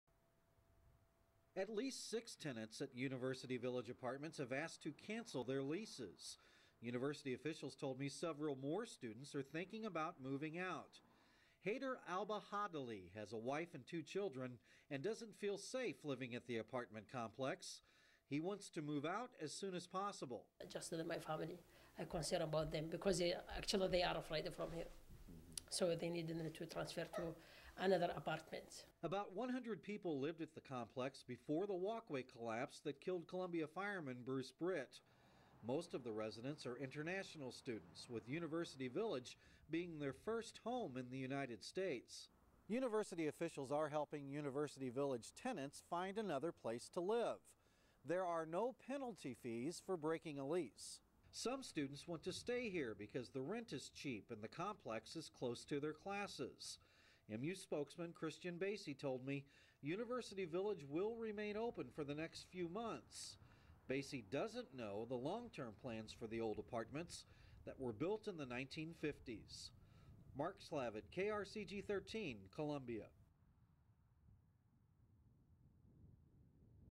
University Village tenants cancel leases
Some Mizzou students want to get out of their leases while school leaders decide what to do about University Village Apartments. That's where Columbia Fireman Bruce Britt was killed last month while responding to a collapsing walkway. KRCG 13's Mark Slavit reports.

KRCG 13